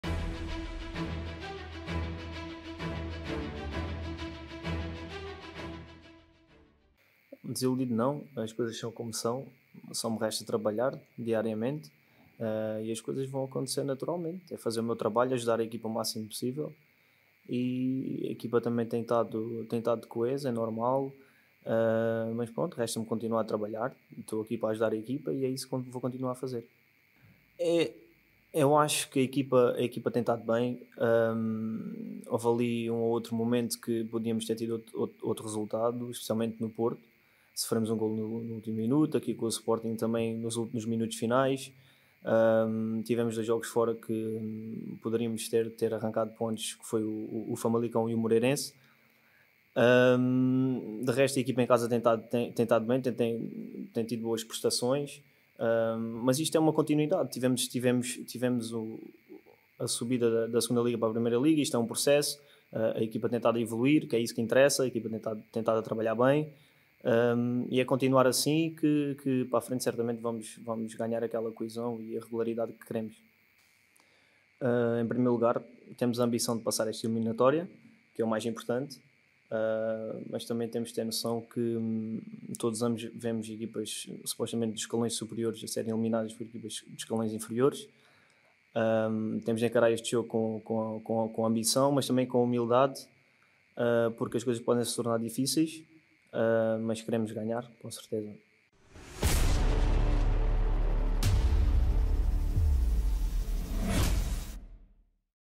𝘼𝙣𝙩𝙚𝙫𝙞𝙨𝙖𝙤 🏆 Taça de Portugal 🟢⚪ Lank FC Vilaverdense x SC Farense ⚫⚪ 🗣️ Vitor Gonçalves
⏳ 15h00
🗓️ 22 de outubro - domingo
📺 sem transmissão